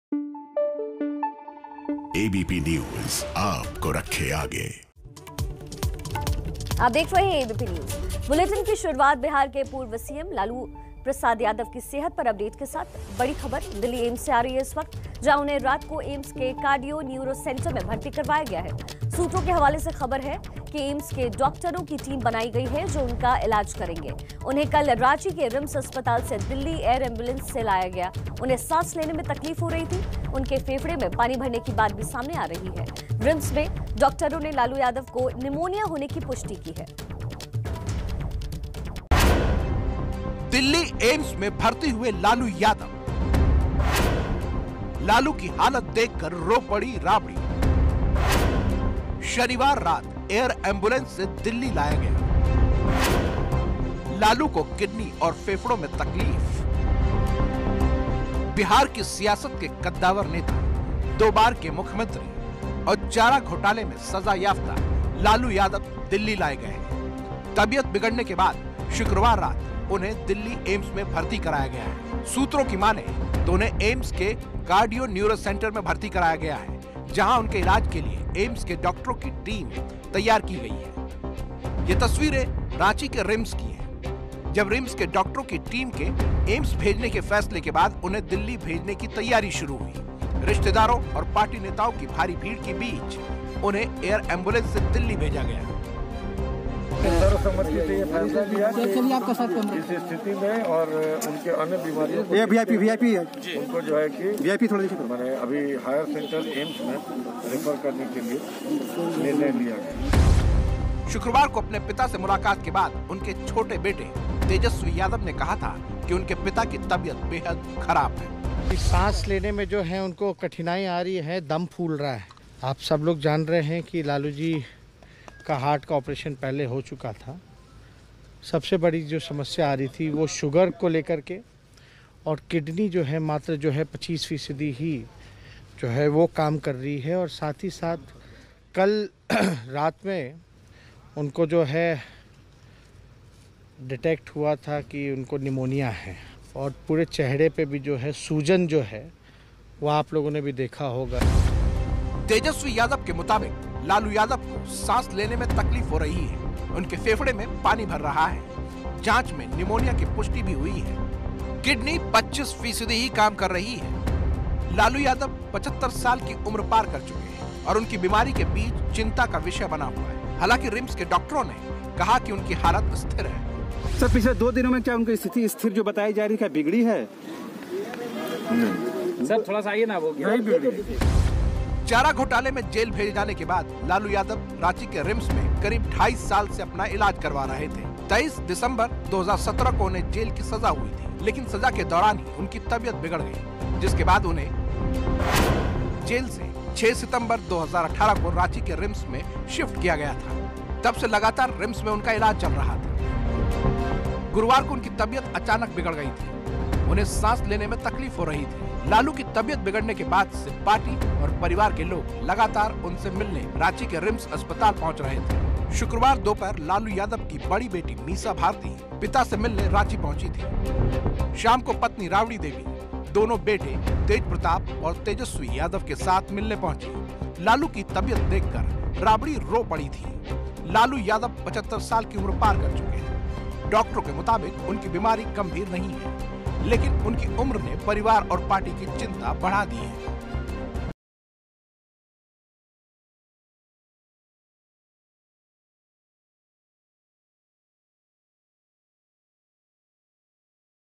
दिल्ली एम्स में भर्ती हुए लालू यादव, निमोनिया होने की पुष्टि, किडनी 25 फीसदी ही काम कर रही
सांस की तकलीफ के बाद एयर एंबुलेंस में रांची से आरजेडी सुप्रीमो लालू प्रसाद यादव को दिल्ली लाया गया है. जहां उन्हें रात को एम्स के कार्डियो न्यूरो सेंटर में भर्ती कराया गया है.